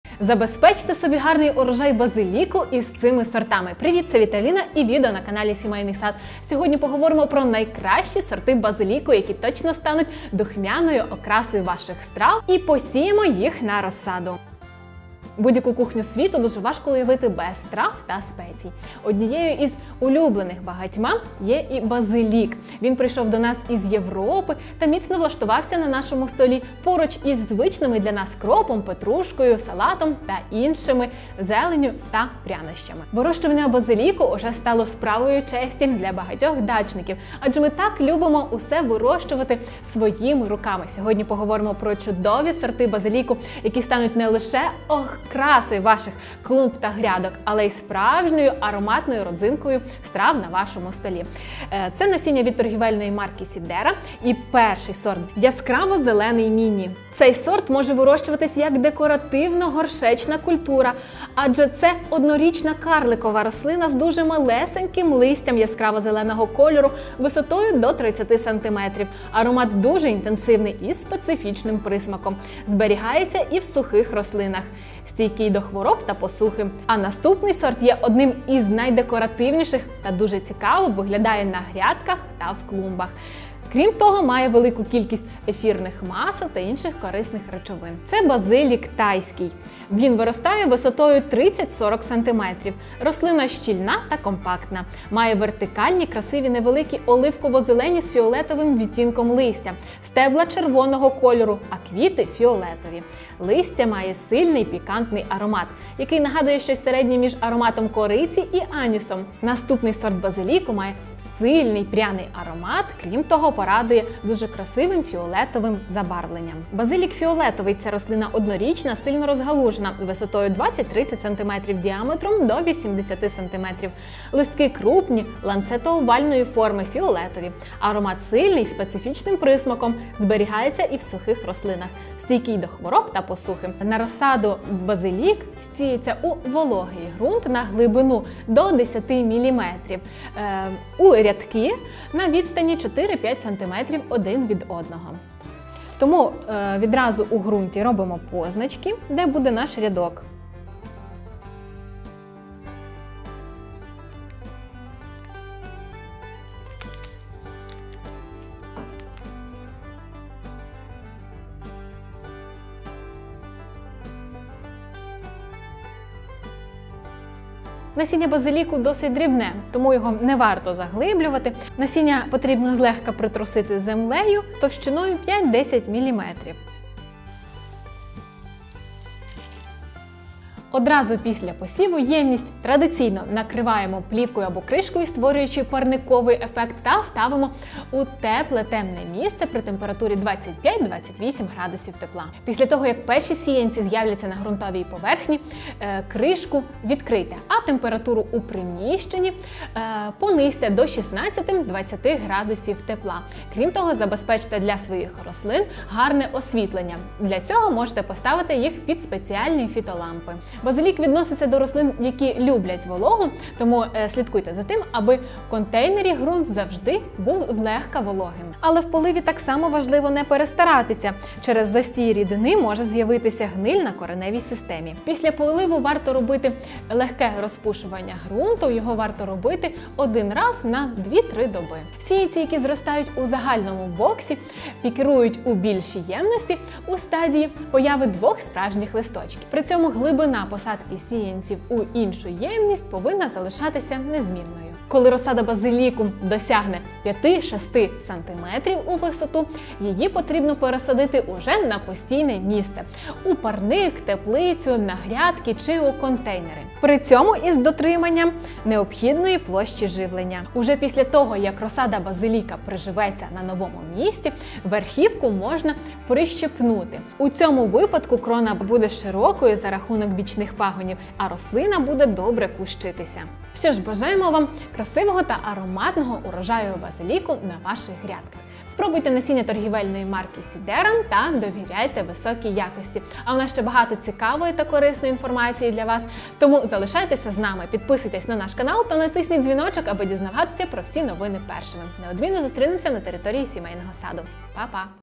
Саме так потрібно вирощувати базилік. Величезний врожай.
З любов'ю, Ваш Сімейний Сад :).